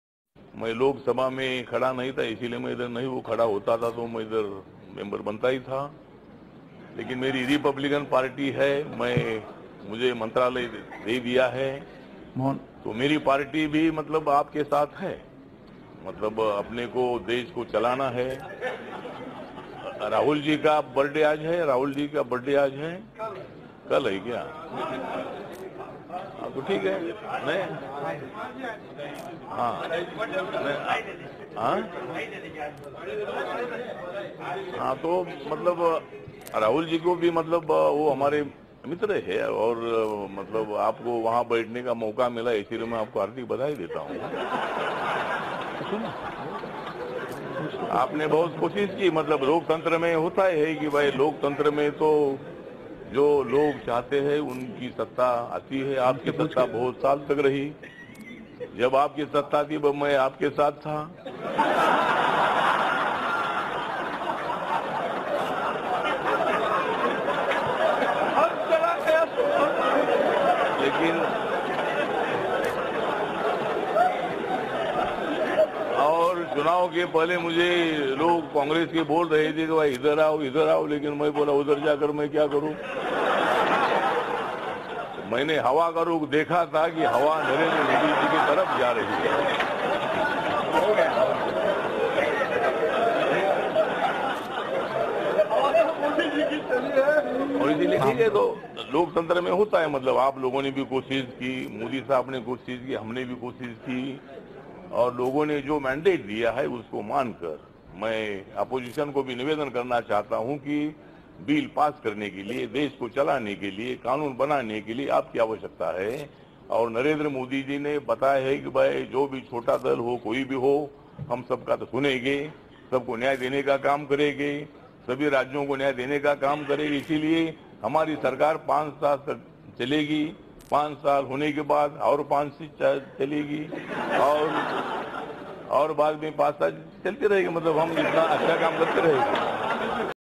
रामदास आठवले यांच्या भाषणाने मोदीसह राहुल गांधीना हि हसू आवरल नाही
रामदासजी आठवले यांनी संसदेत आज असे भाषण केले की
भाषण ऐकून पंतप्रधान मोदीसह राहुल गांधी पण हसत राहिले 😅